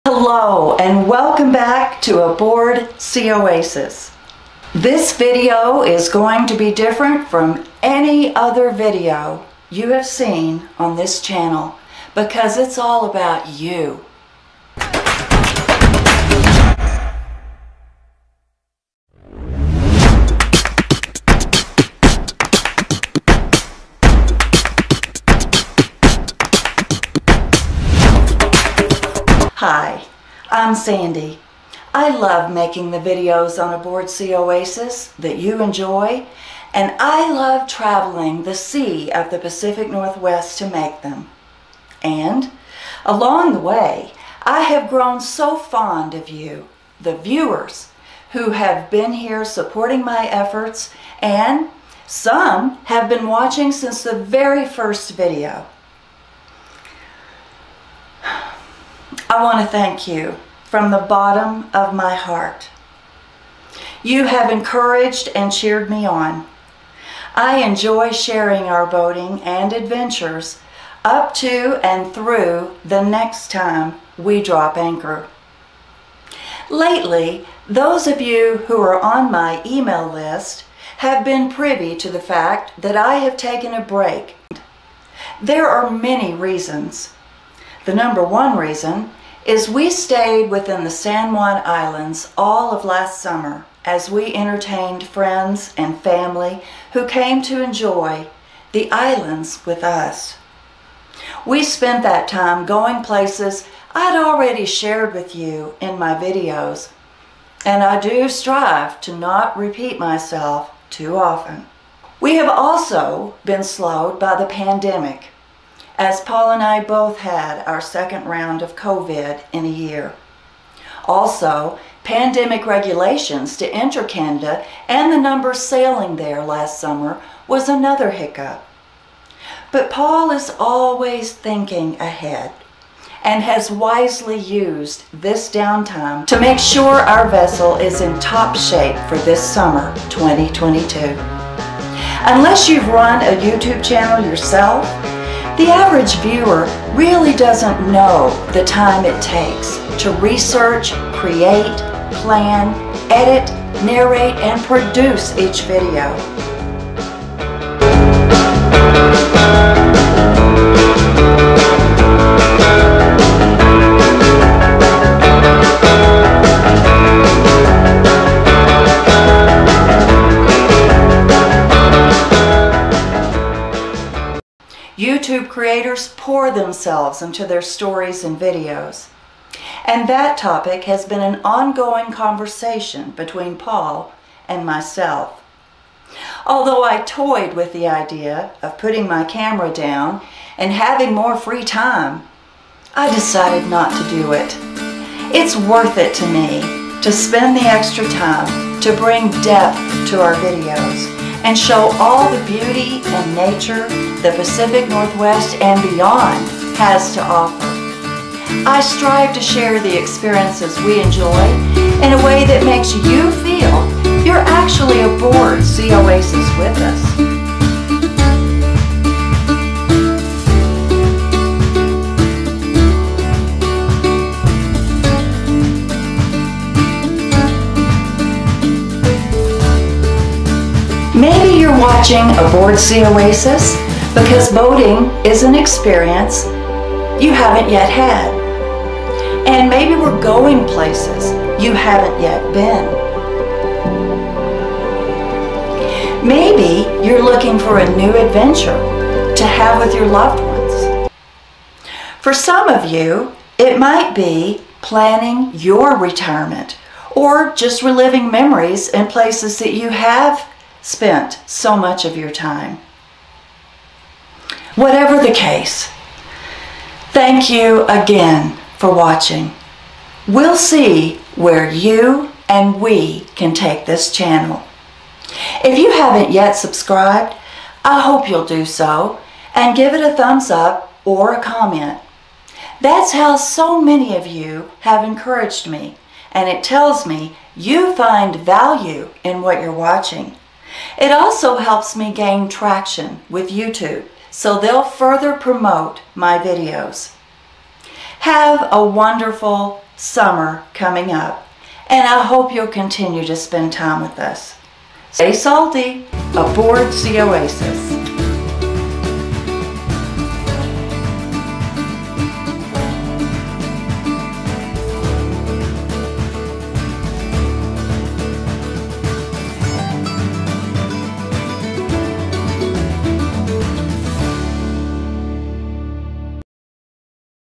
We've Been Silent and a Heart-Felt Thank you!
This video is for our viewers - we've been silent and want to first thank you for your loyalty to our channel.  Then we'll share some information that will explain our silence.  We really appreciate you and want to be candid about what's been happening.